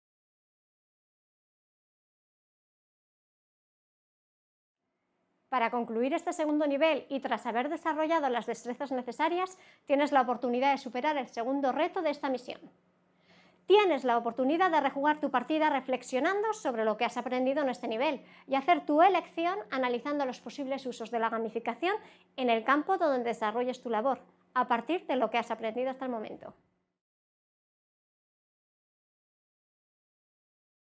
Vídeo 2.7. "Dinámicas, motivación y jugadores" - Reto
Vídeo 7 de la Unidad 2 del MOOC de AprendeINTEF "Gamificación en el aula" GamificaMooc. Para más información, visita